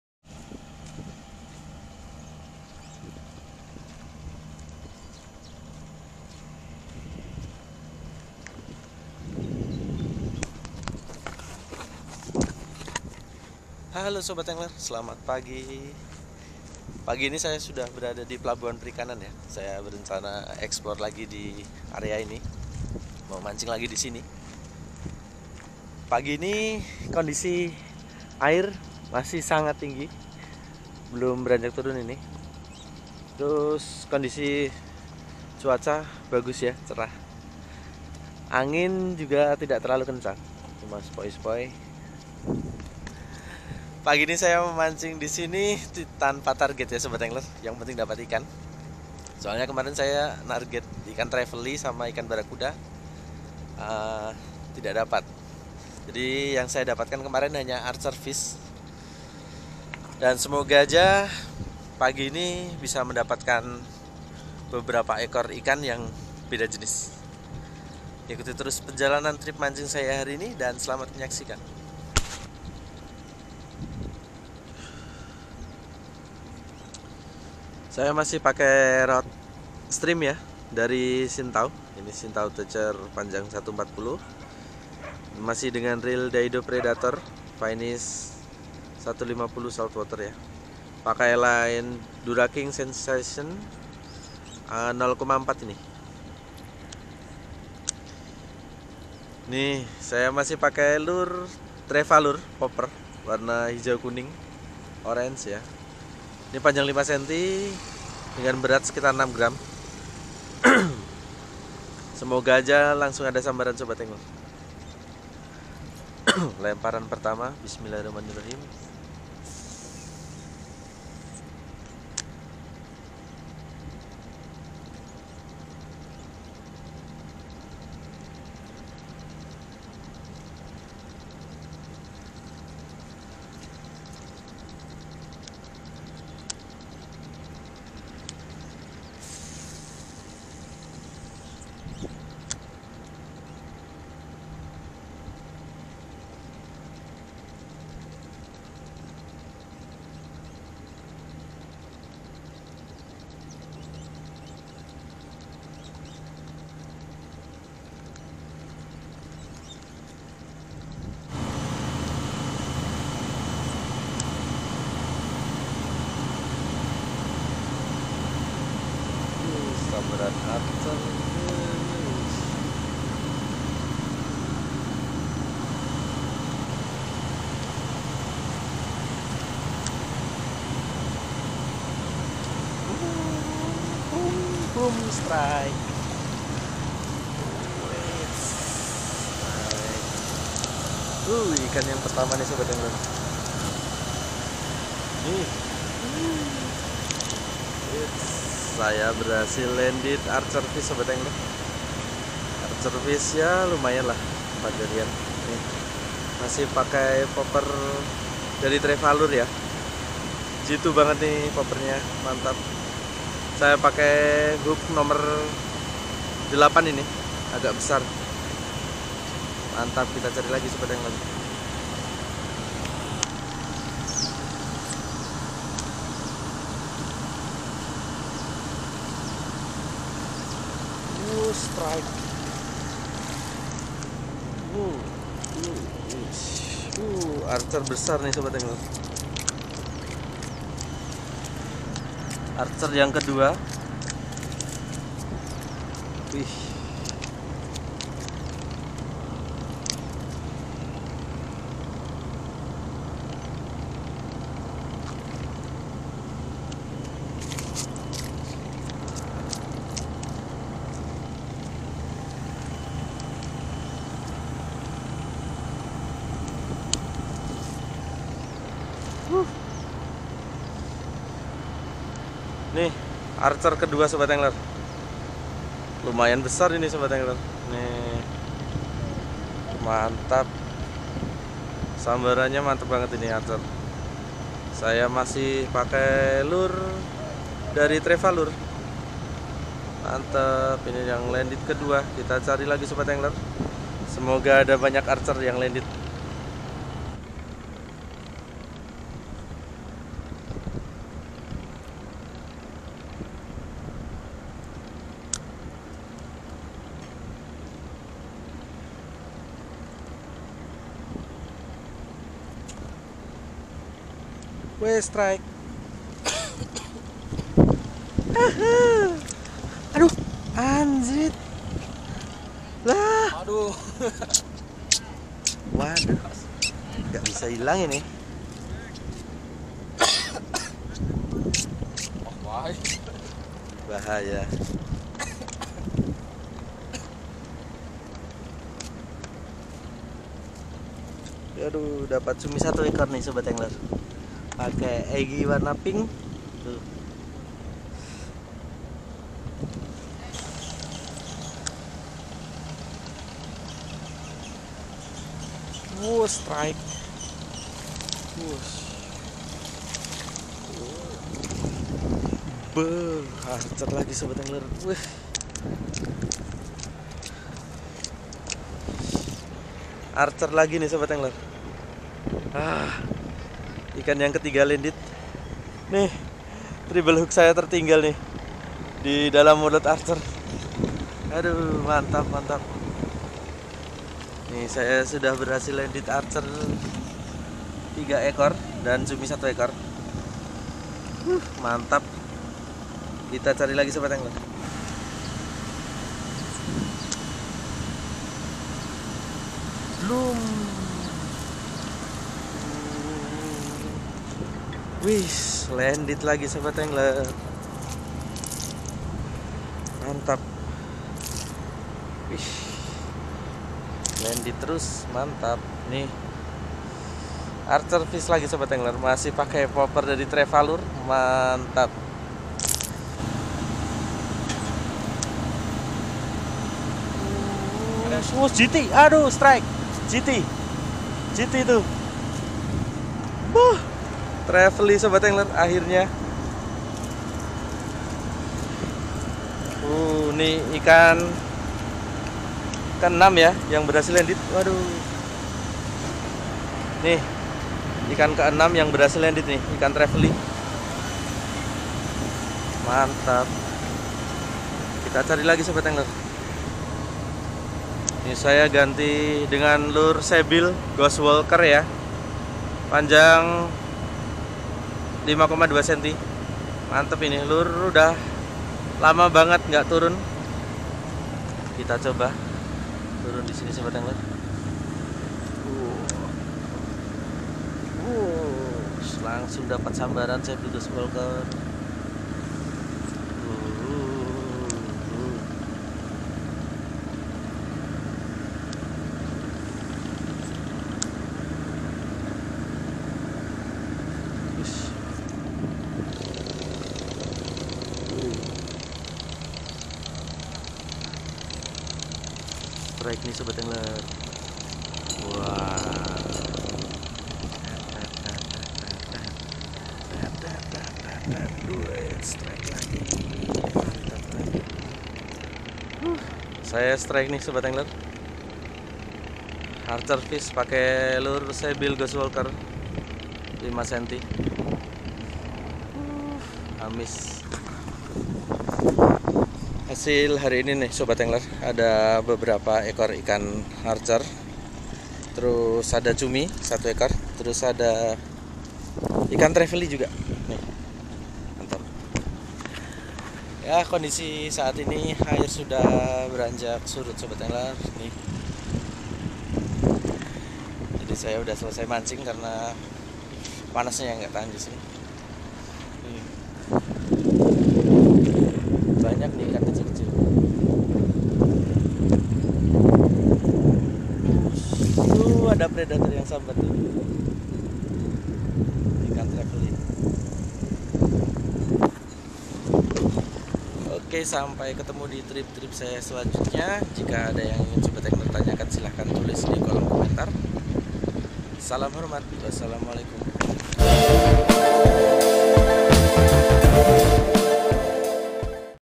Sambaran Brutal Ikan Pemanah Dengan Rod Fiber Xintao Teucer 140 & Daido Predator Finesse 150 SW
Mancing di Pelabuhan Perikanan Kota Dobo

Rod  : Xintao Teucer 140 BC
Reel : Diado Predator Finesse 150 SW
Line : Duraking Sinsation 0.4
Lure : Popper Trevalure 5cm, Sebile Ghost Walker 5.2cm